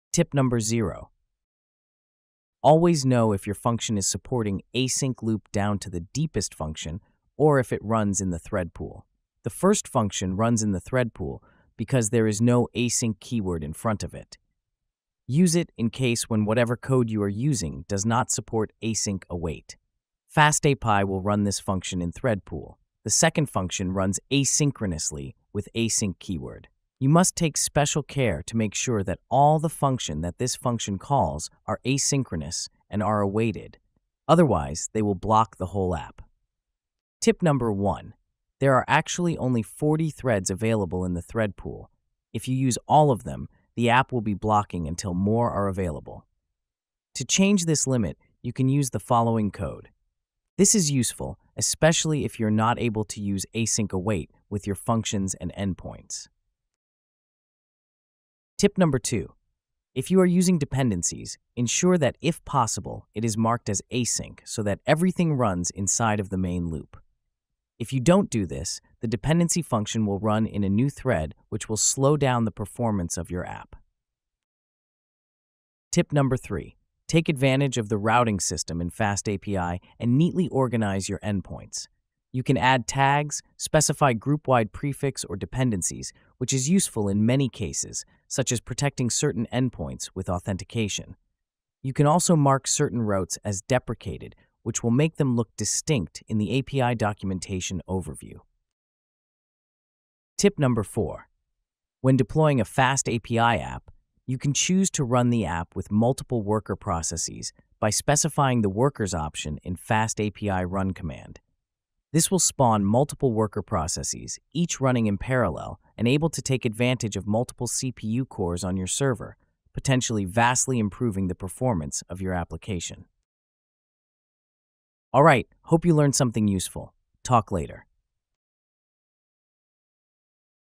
5 FastAPI ProTips For Writing Better API
Five quick tips to make your Fast API app work faster and better.